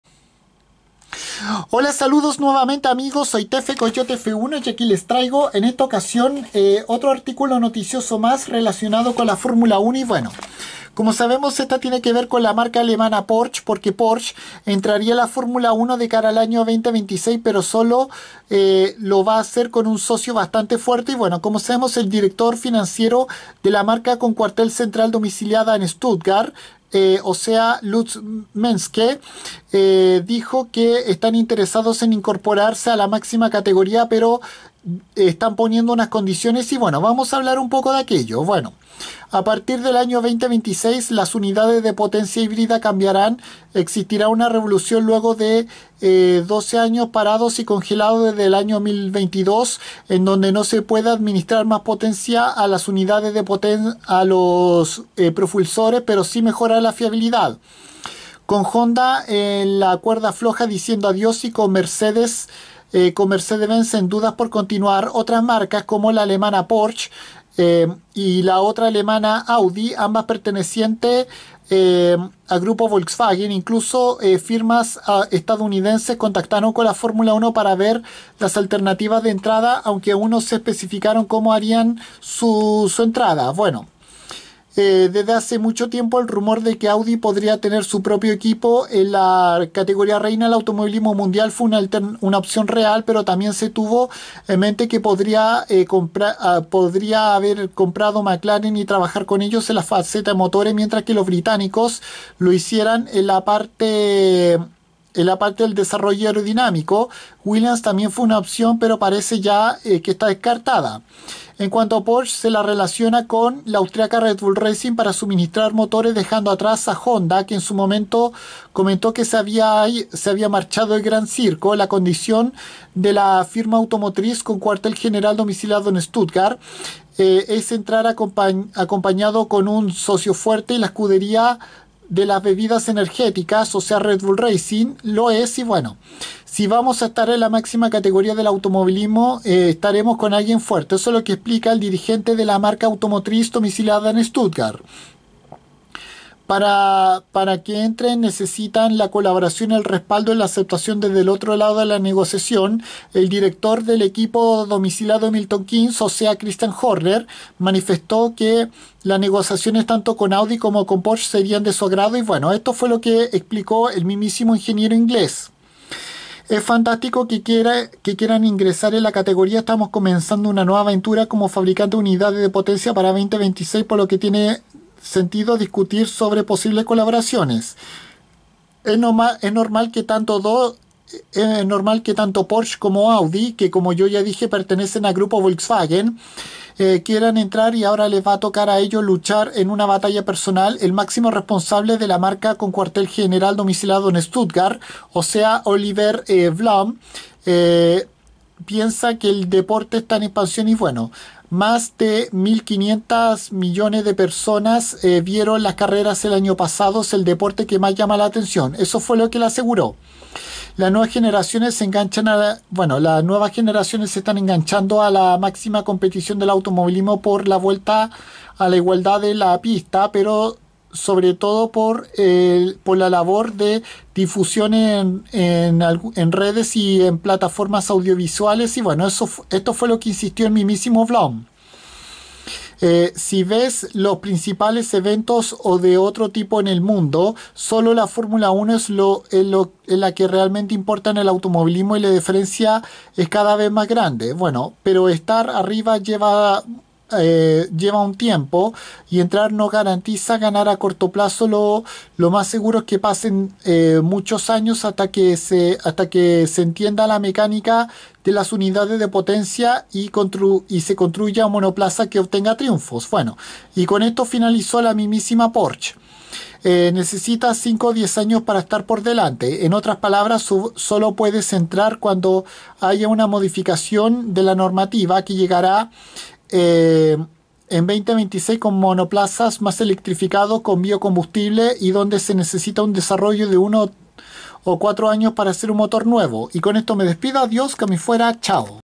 Porsche entraría a la F1 en la Temporada 2026 solo con un socio fuerte
La firma automotriz de Stuttgart pone algunas condiciones a la categoría reina del automovilismo mundial con el propósito de ingresar a la Temporada 2026 con el cambio reglamentario de las unidades de potencia y la introducción del biocombustible